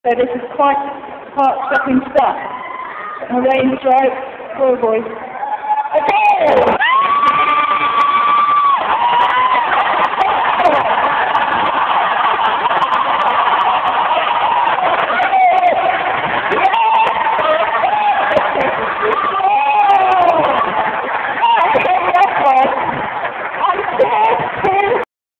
Chariots of fire stunt
At the equine fair chariotsof fire were there this is them jumping over 4 people